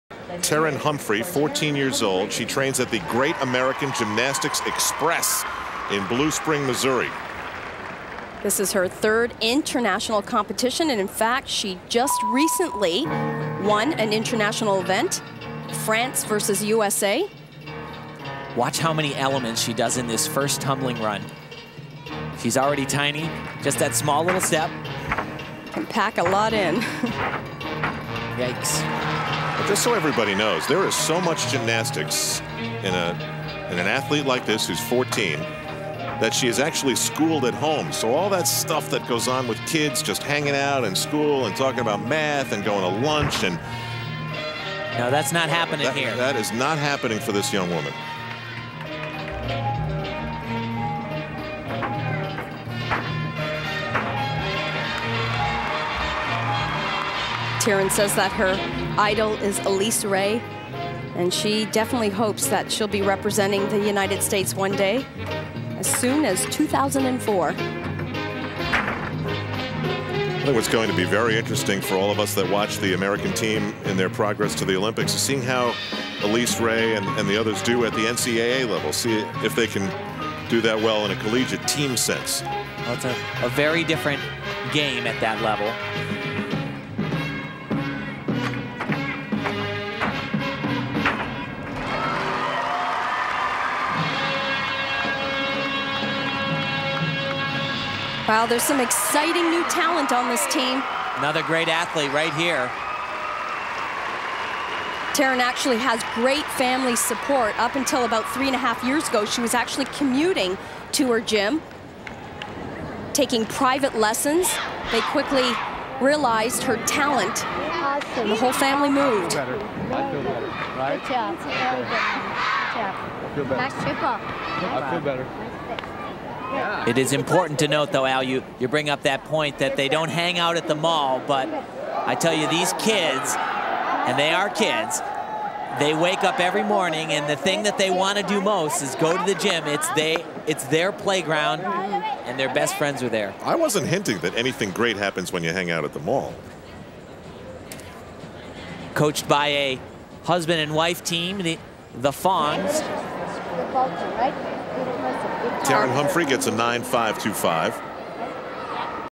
Terin Humphrey - Floor Exercise - 2000 Pontiac International Team Championships - Women
Score: 9.525. Terin Humphrey (USA)
Nov. 10, 2000 - Oncenter - Syracuse, N.Y.

Final Team Standings
1. USA - 151.737
2. Romania - 151.249
3. China - 148.811